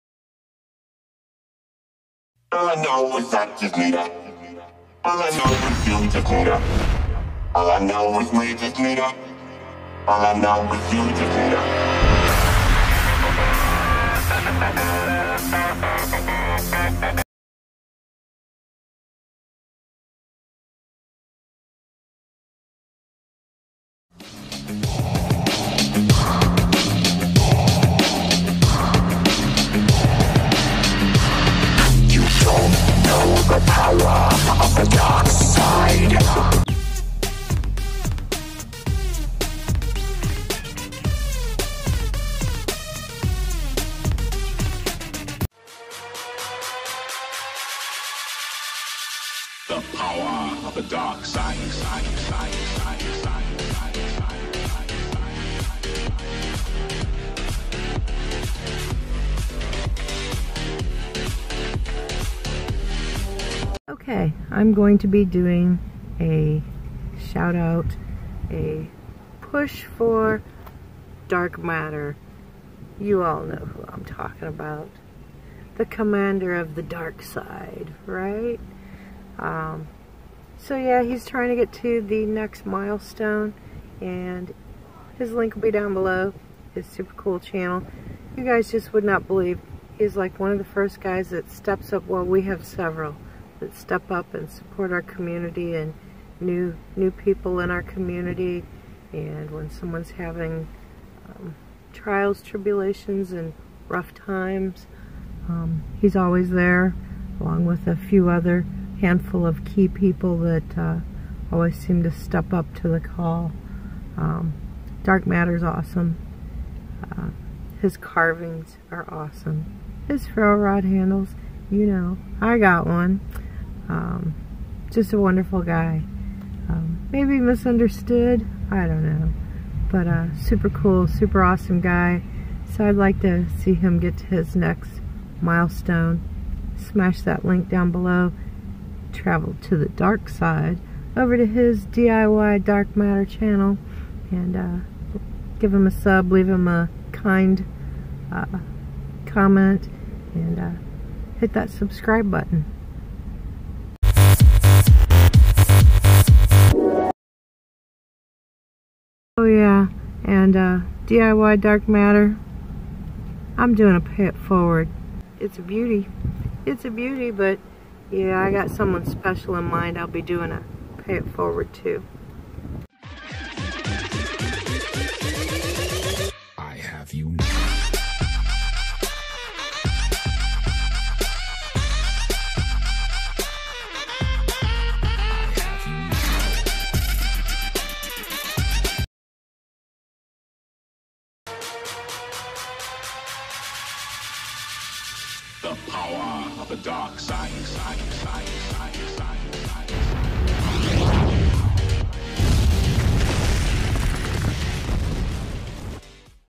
VR for DIY DarkMatter (support video)
Who's our buddy?
Who's our friend?
He's DarkMatter!
Hellloooooooooooooo Everybody

From Commander of the Darkside in BonesYard ArmyManWars
To community support, special projects,
Some good looking FOOD
To just an all around great guy
And...he is has one heck of a sense of humor

Here is
My support video for DIY DarkMatter

And since i already have the best DarkMatter ferro rod ever.
I will be doing a PIF if I should by chance

Thanks Dark!
Stay Awesome!